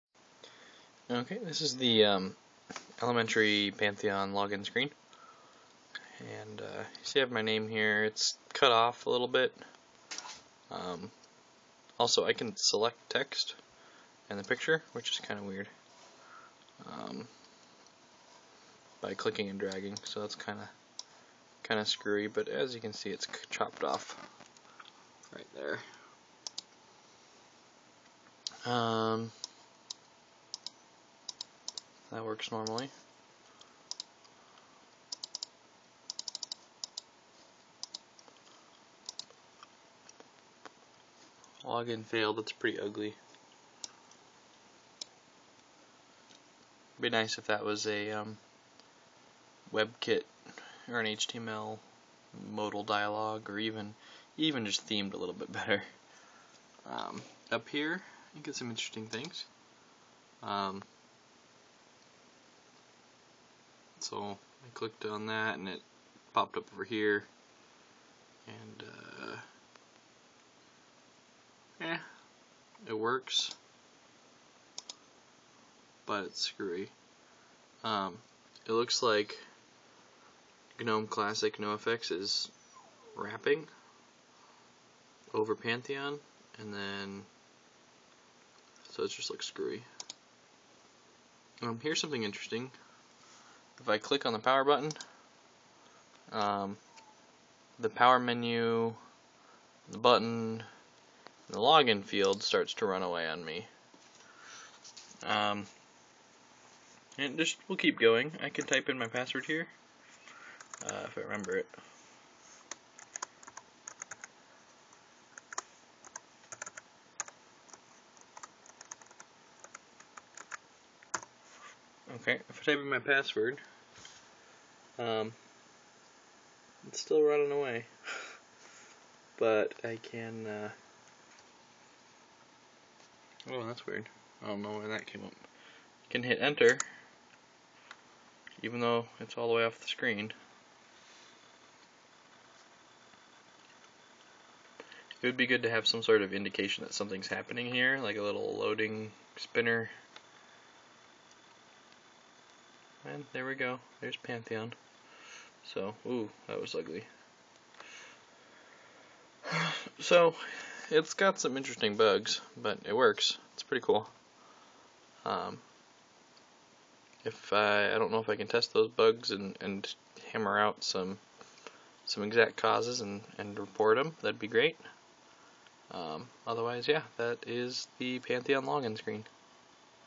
elementary Pantheon Login Screen
A quick look at an experimental build of the elementary login screen. It's far from finished and has some pretty evident bugs.